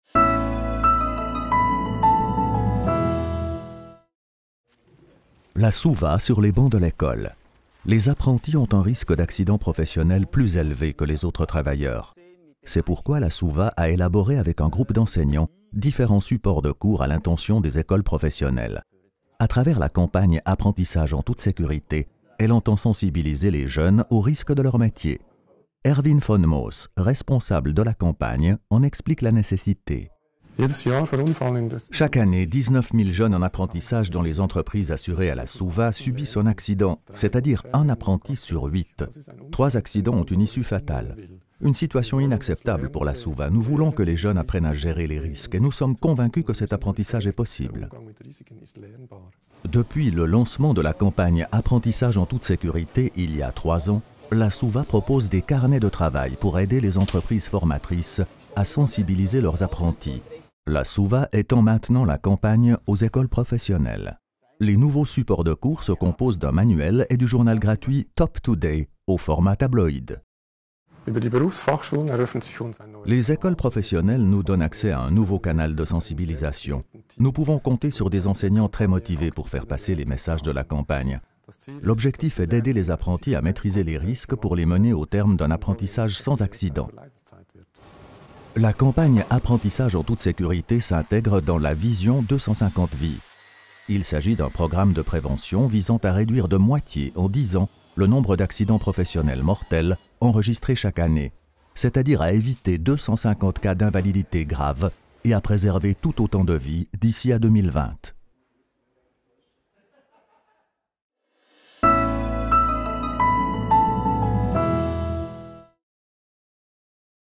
Conférence de presse 2016: La Suva sur les bancs de l'école
– Les apprentis ont un risque d'accident professionnel plus élevé que les autres travailleurs. La Suva a élaboré, avec un groupe d'enseignants, différents supports de cours pour que les écoles professionnelles puissent sensibiliser les jeunes et leur faire prendre conscience des risques de leur métier. Le journal gratuit «top TODAY» au format tabloïd en constitue la pièce maîtresse.

La Suva est plus qu'une assurance : elle combine la prévention, l'assurance et la réhabilitation.